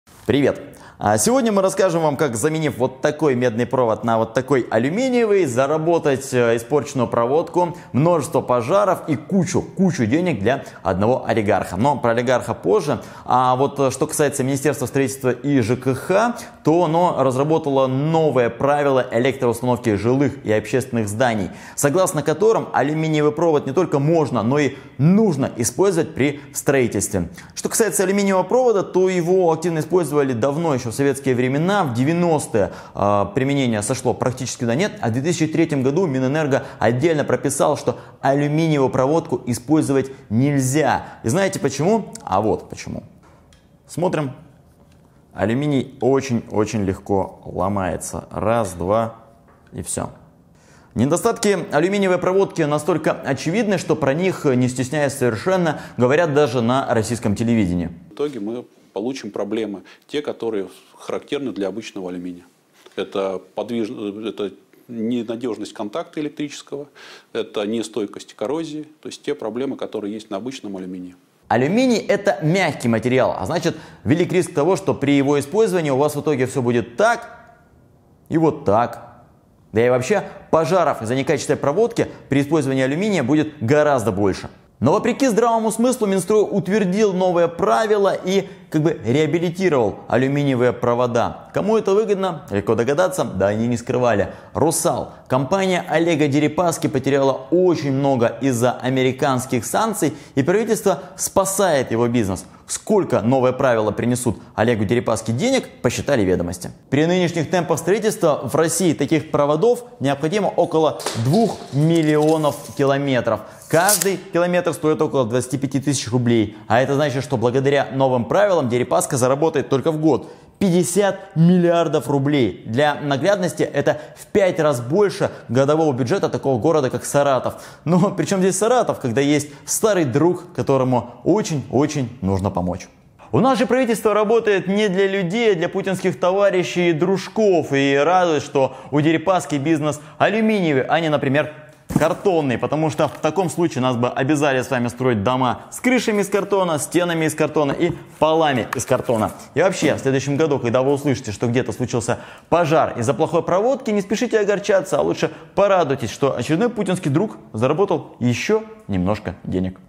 Пожар в каждый дом. Как Дерипаске чинили бизнес
Российские власти придумали ещё один способ, как увеличить капиталы олигарха Олега Дерипаски. Дерипаска серьёзно пострадал от американских санкций, а это значит, что пришло время компенсировать его убытки за счёт россиян. Пусть даже ценой пожаров в их домах.

Минстрой решил вернуть в новые дома алюминиевую проводку, которая не применялась со времён Советского союза. Алюминий обладает повышенной текучестью и плавится при высоких нагрузках. В правительстве об этом знают, но олигарх им важнее. Смотрите новое видео канала «Штаб» и вы узнаете, чем Дерипаске помогут изменения строительных правил и сколько десятков миллиардов они ему принесут.

Автор — Дмитрий Низовцев